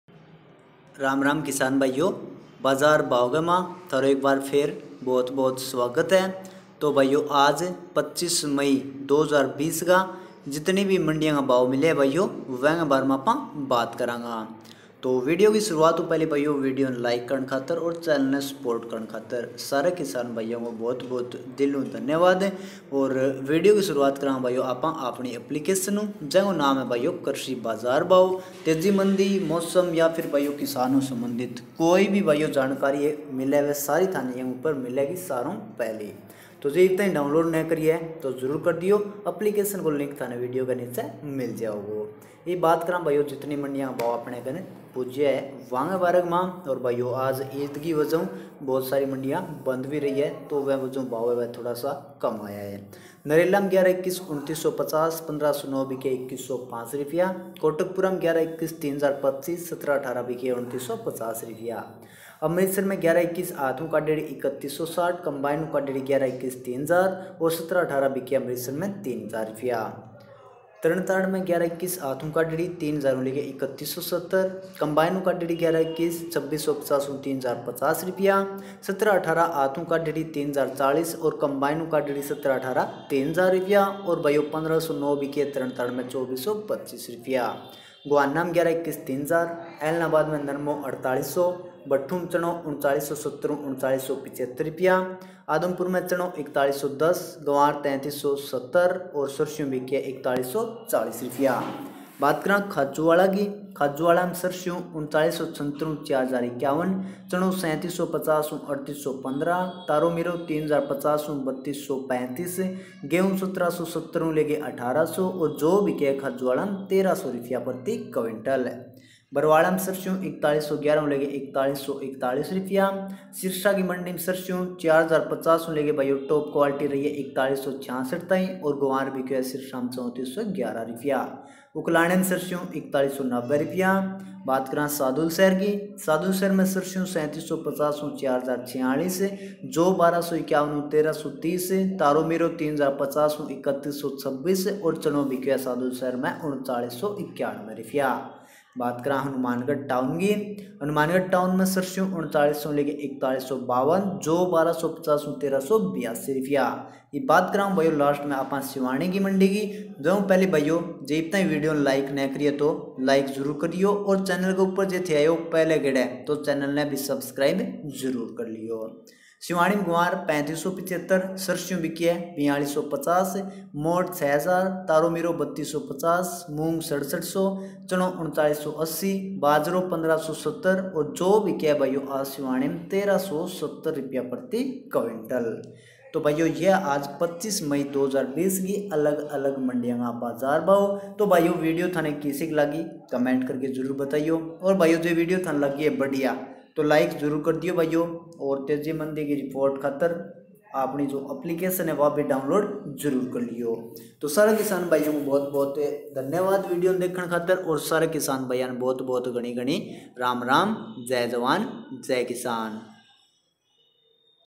25 मई 2020 मंडी भाव । धान के आज के भाव । सिवानी मंडी भाव । सादुलशहर मंडी भाव । आदमपुर मंडी भाव
Live मण्डी भाव, मौसम और तेजी मंदी  के लिए निचे दिए लिंक पर क्लिक करें 👇👇👇